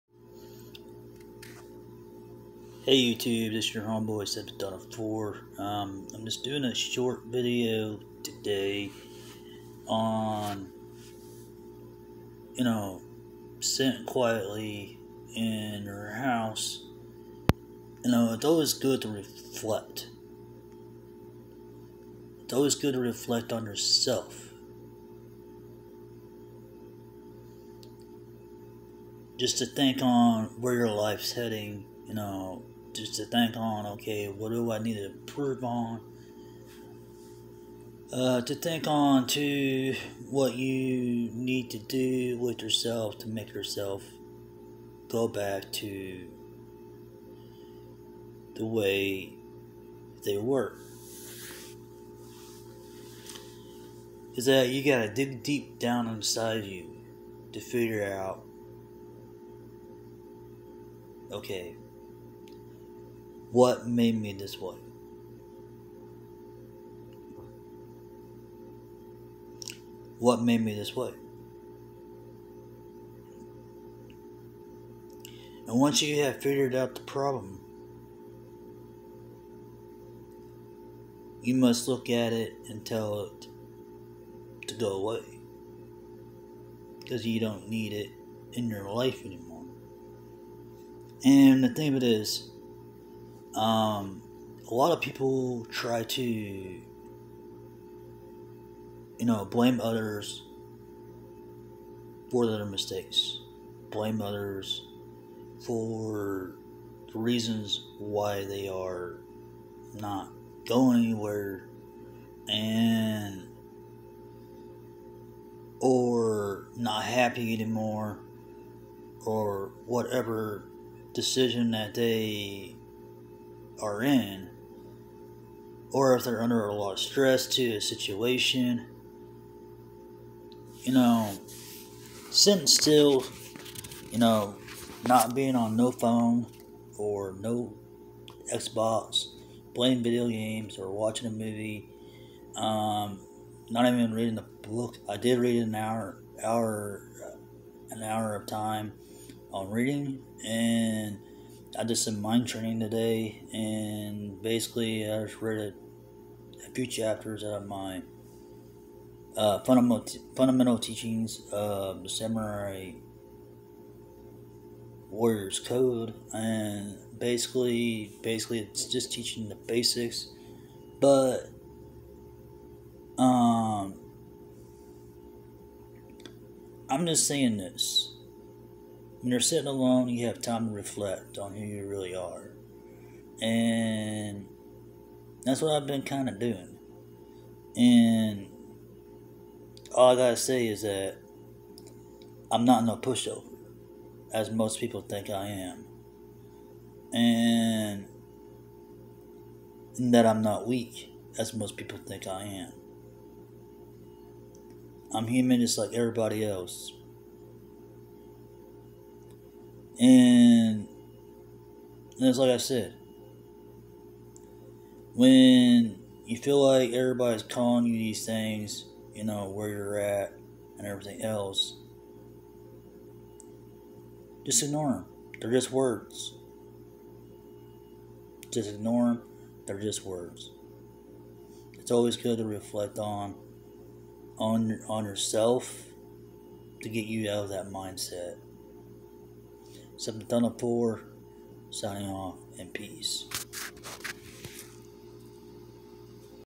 Reflect On Yourself Is Always Good!
Reflect on Your self is always a good thing because you can always try to figure out what made you that qay in the first place. If people calls you names just ignore them if people calls you weak just ignore them. Its always good to reflect on your self to find out who you really are.